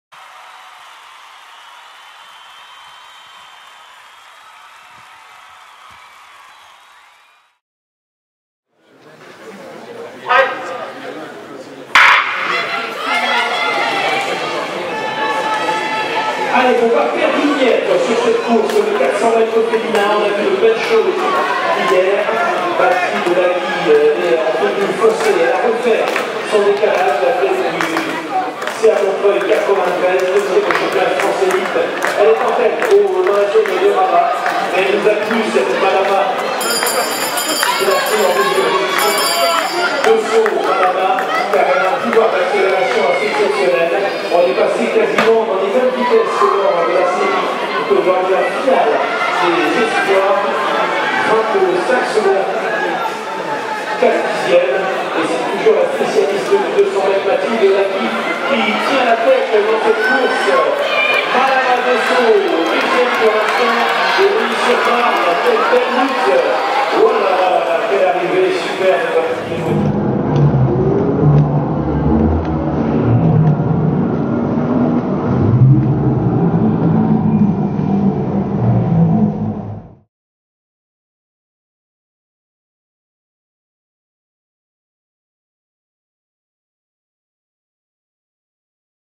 400m Femmes Championnats Nationaux
Championnats de France Espoirs & Nationaux en salle - Rennes - 20 et 21 Février 2016